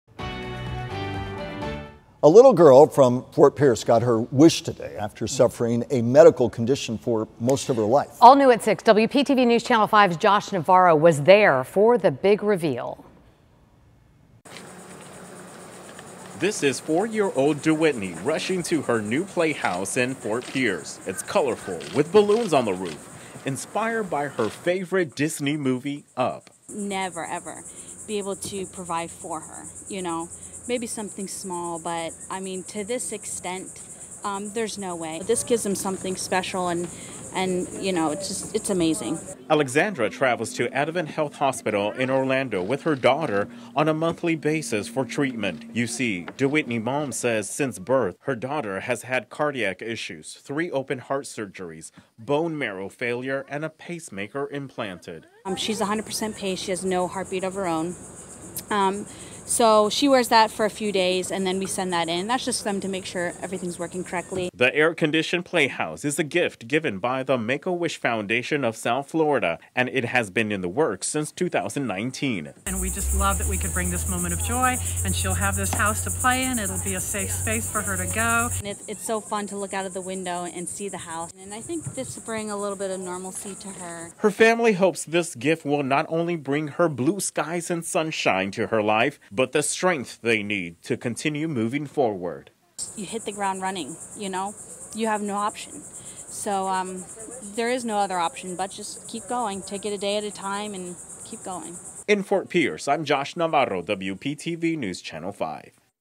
Make-A-Wish Southern Florida gives 4-year-old girl new playhouse
Tuyeet Nhi received an "Up" themed playhouse from Make-A-Wish Southern Florida.